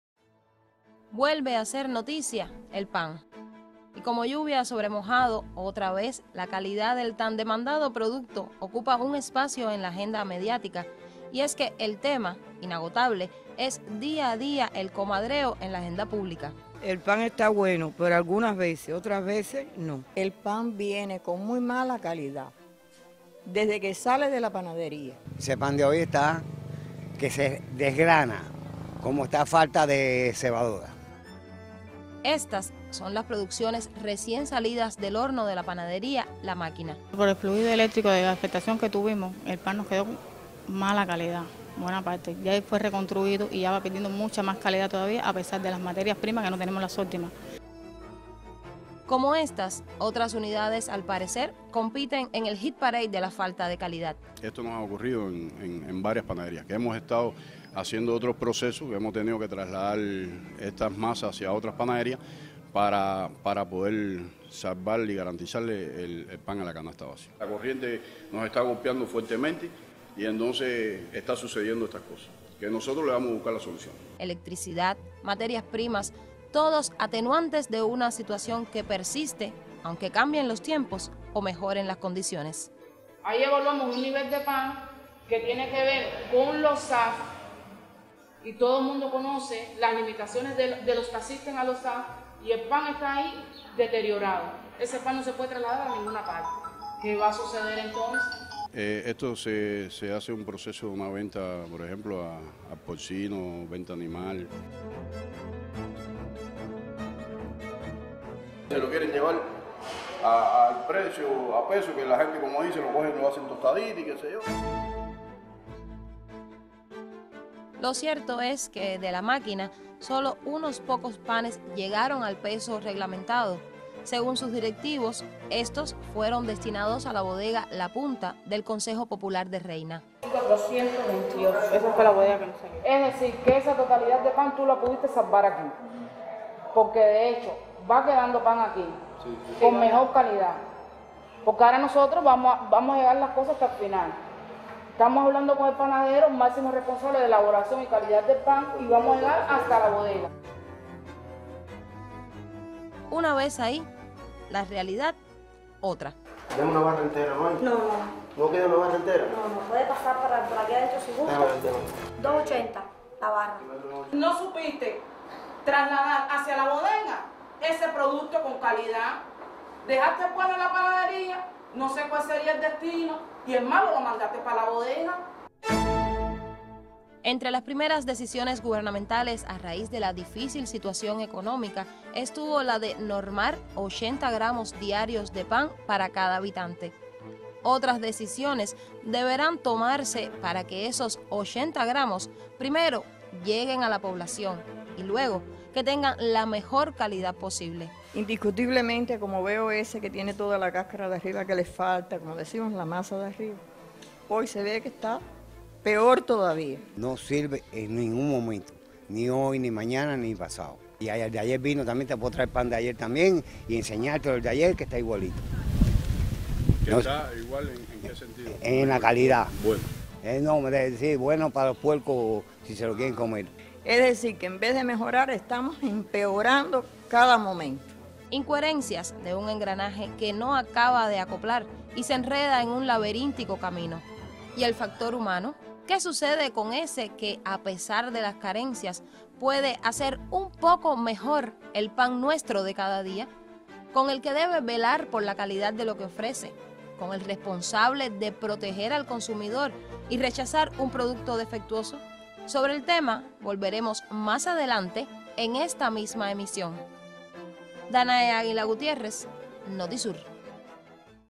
Acciones para erradicar corrupción e ilegalidades en Cienfuegos
Un control a panaderías del municipio de Cienfuegos, detectó deficiencias en la producción de la unidad “La Máquina”. Inspectores, junto a representantes de organizaciones políticas accionaron allí, ante las problemáticas encontradas.